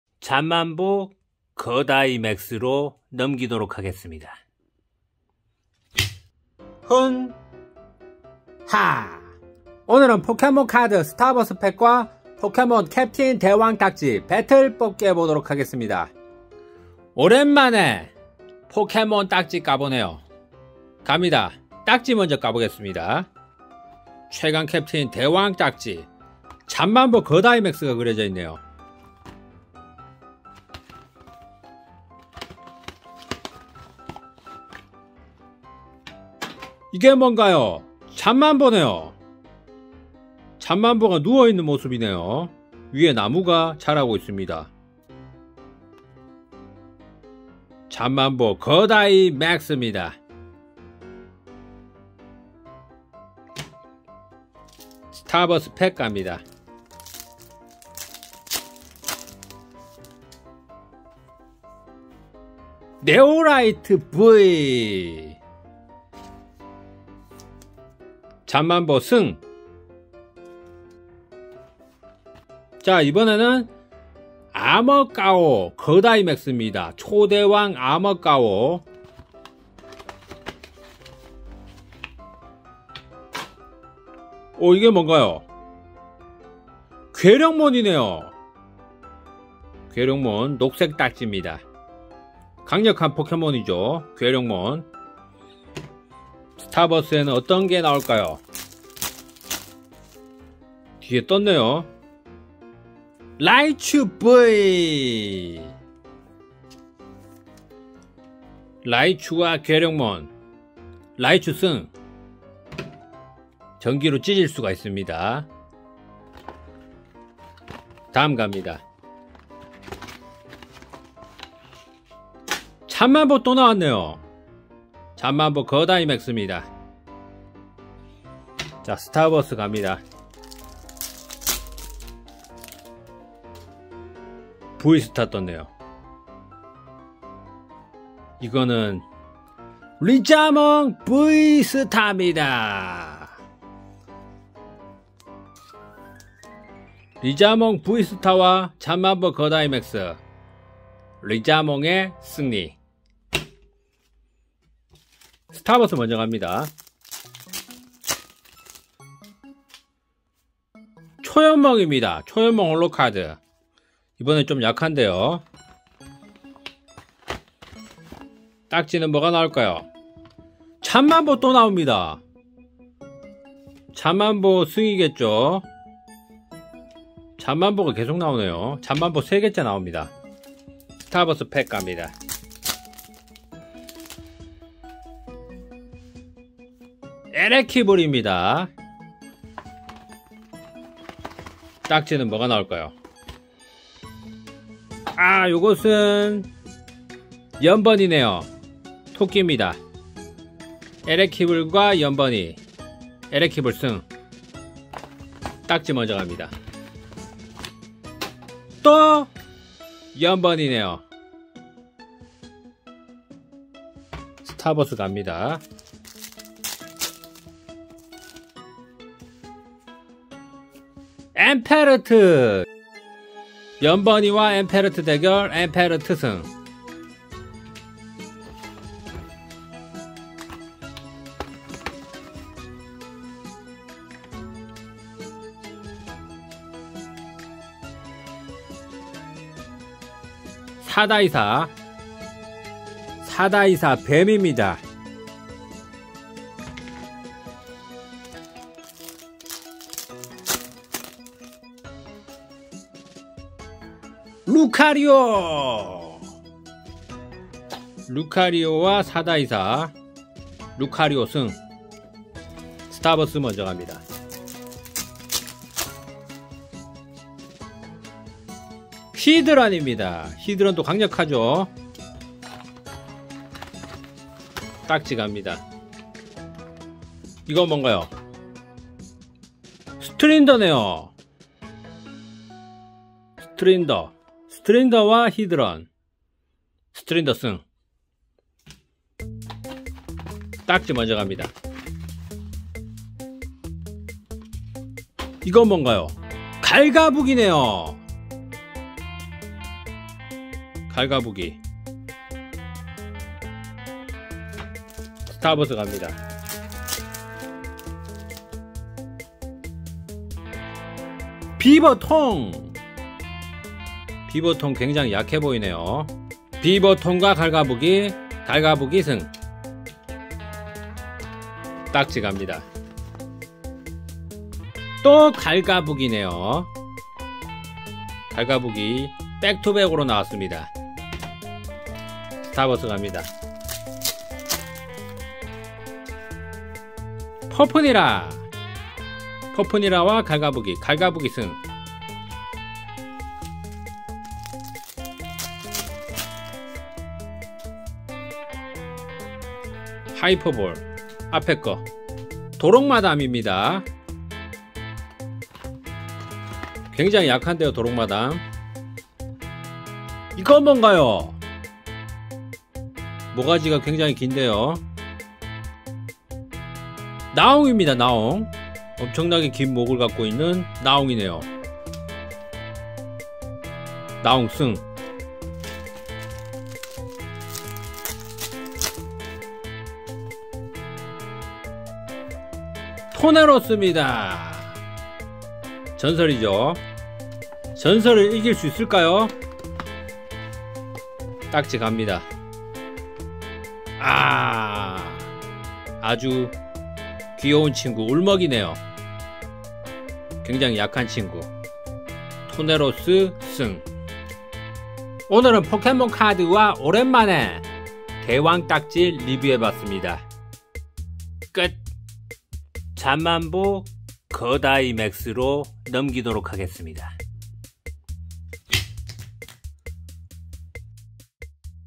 포켓몬카드 스타버스 VS 포켓몬 대왕딱지 뽑기 대결
포켓몬스터 카드 소드&실드 확장팩 스타버스 vs 포켓몬스터 캡틴 대왕 딱지 배틀 뽑기 대결